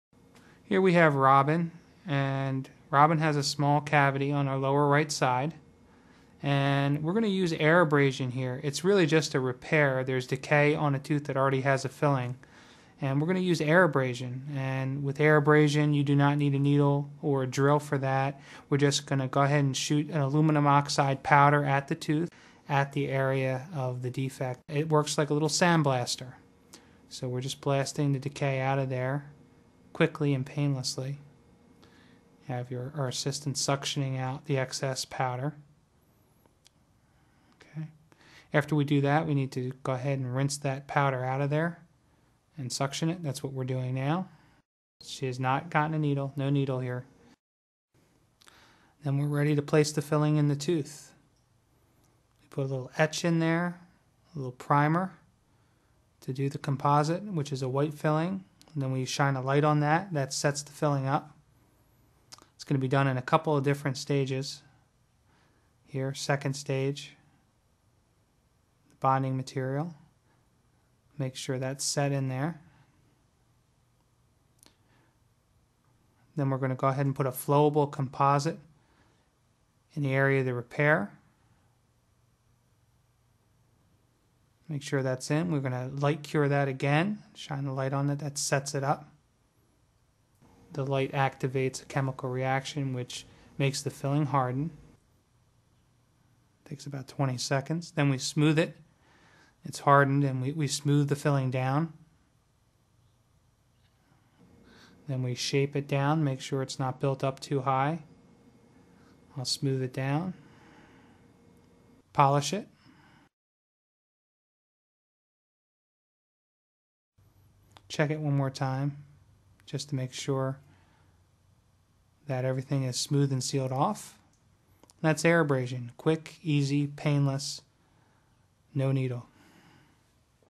Air Abrasion Demonstration
Dr. Jerry Gordon demonstrates the painless air abrasion technique on one of his patients.  He also describes the procedure.